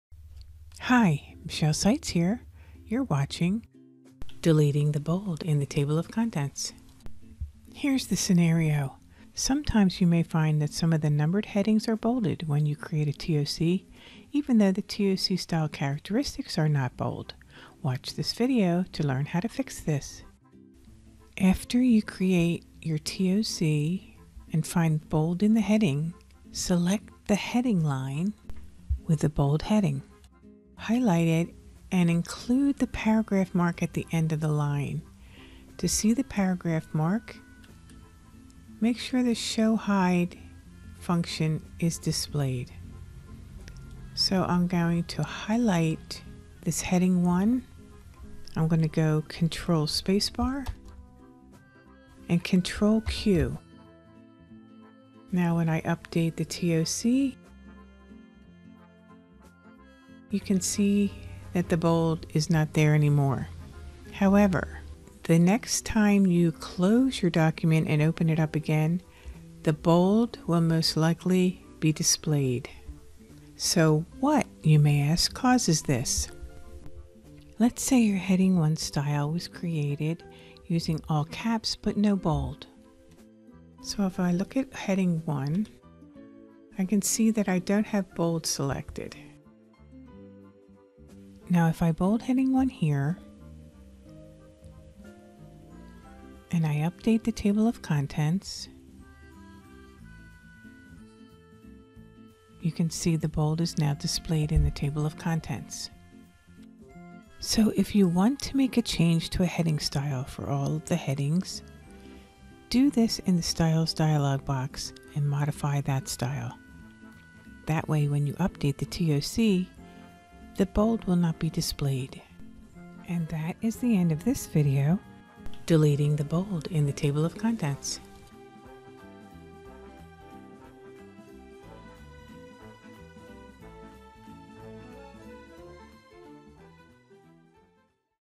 Deleting the bold in the TOC
Sometimes when you create a Table of Contents, there are heading levels that may have bold in them. However, the TOC styles in the Styles dialog box are NOT bold. Why does this happen?  Watch this video to find out!